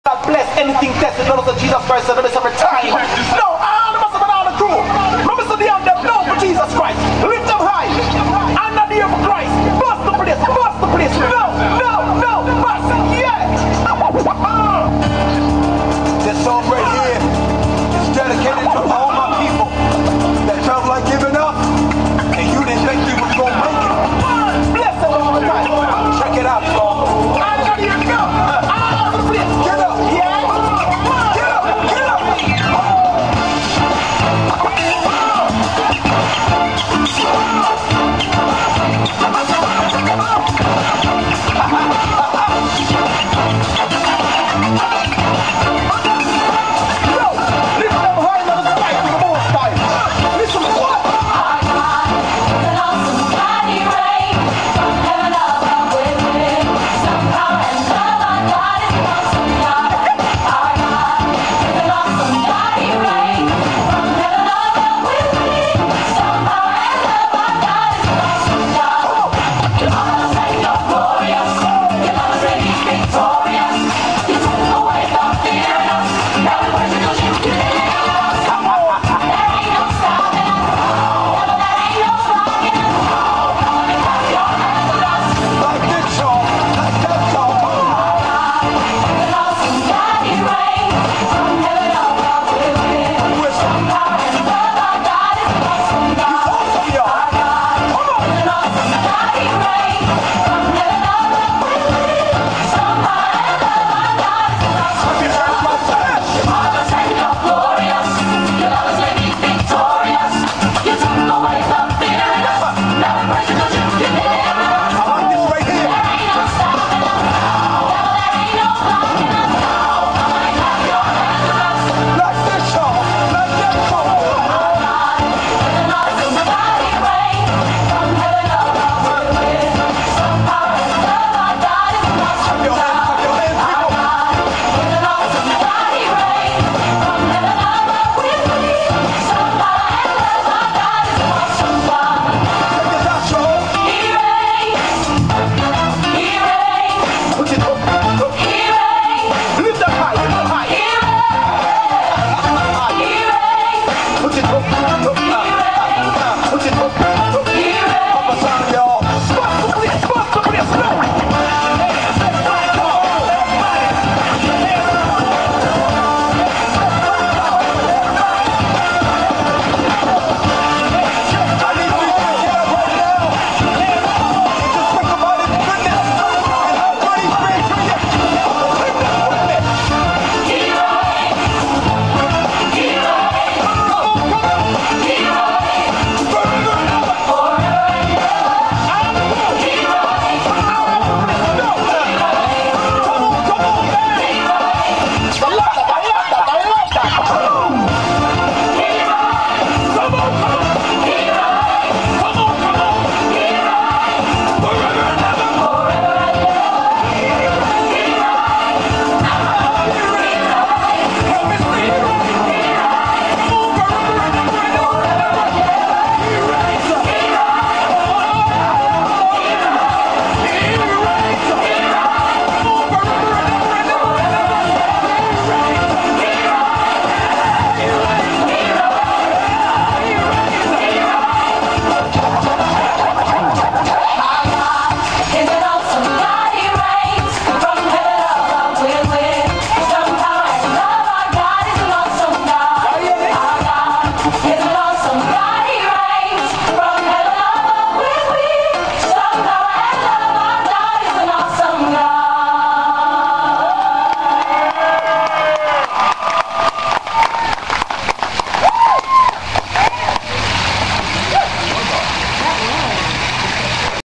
2010 City Church Easter event
April 3, 2010: City Church of Fort Myers, Florida out reach event at the Edison Mall.